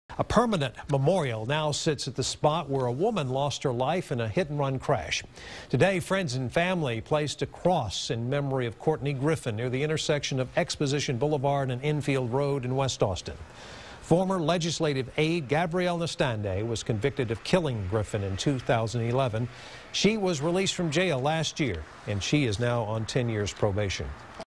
Permanent memorial in place for Courney Griffin
A permanent memorial now sits at the spot where a woman lost her life in a hit and run crash.